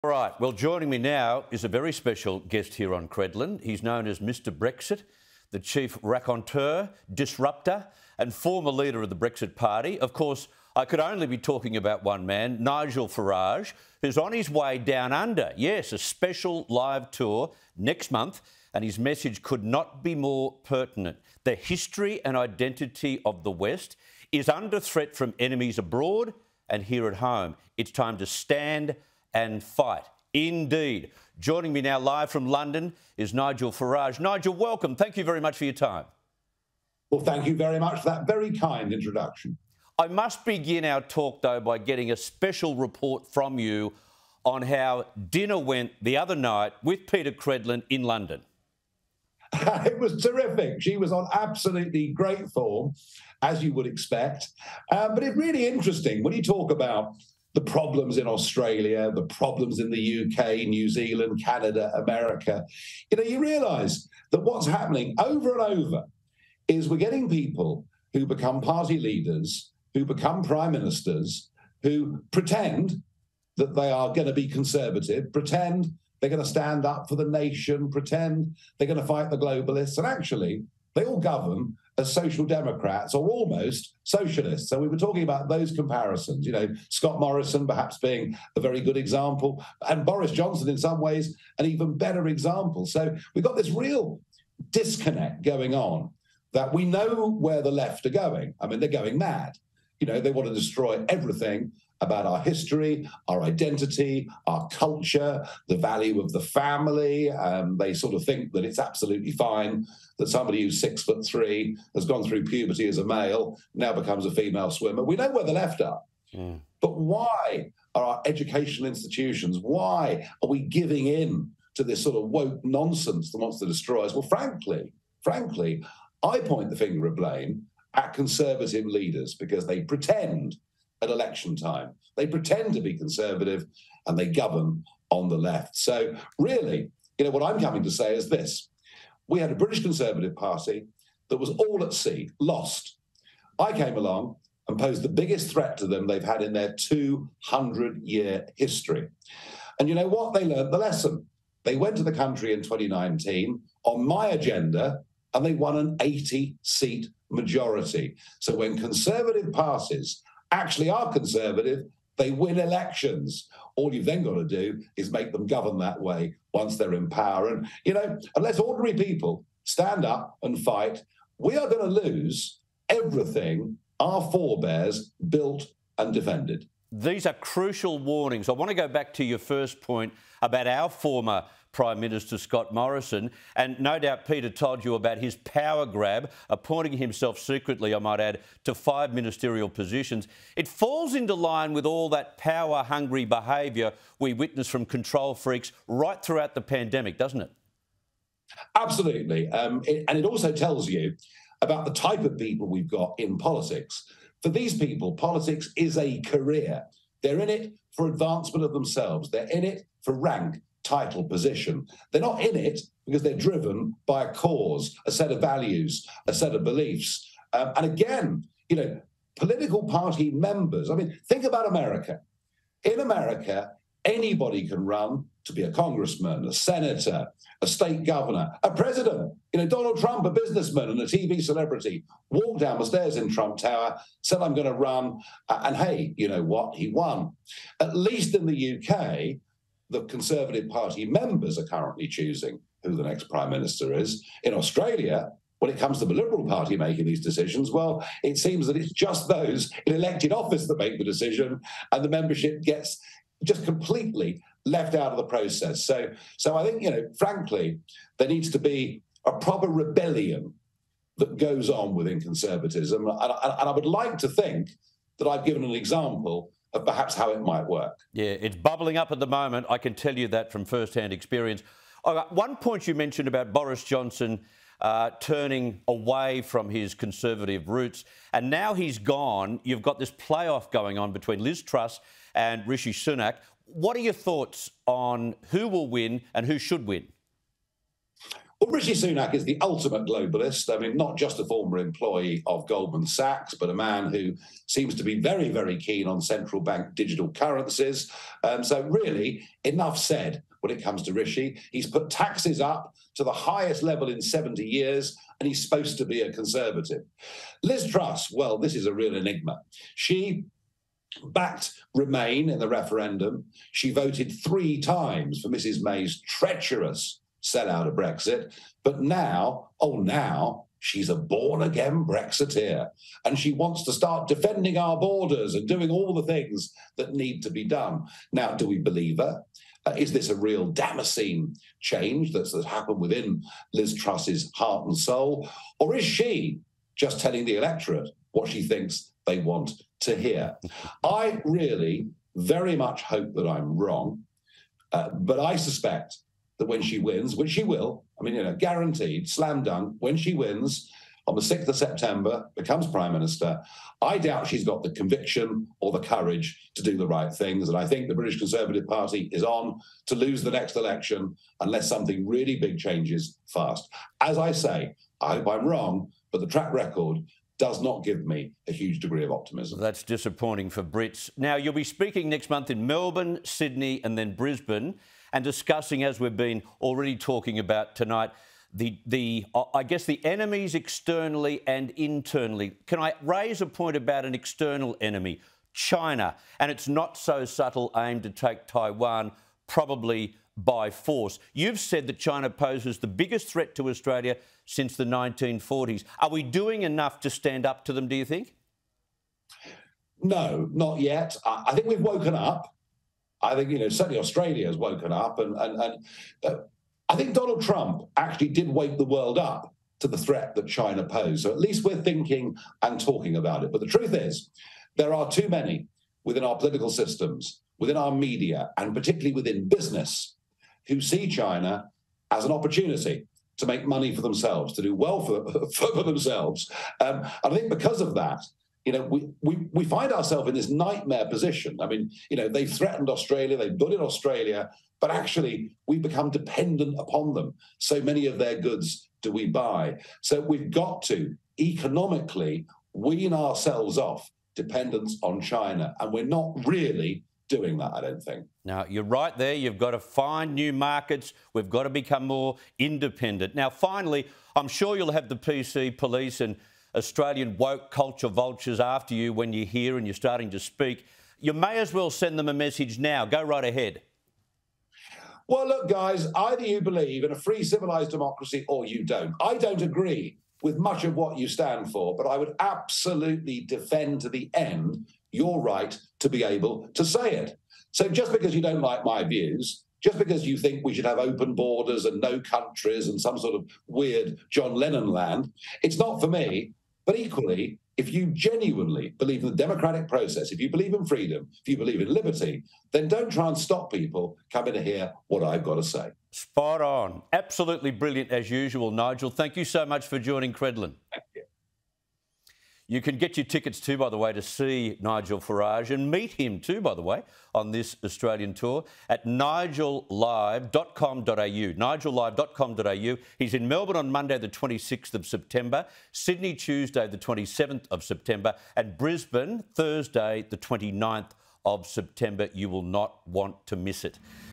Left are ‘going mad’
UK Broadcaster Nigel Farage has questioned why the world is “giving in” to “woke nonsense”.

“I mean, we know where the left are going – they’re going mad,” he said.

“They want to destroy everything about our history, our identity, our culture, the value of the family.”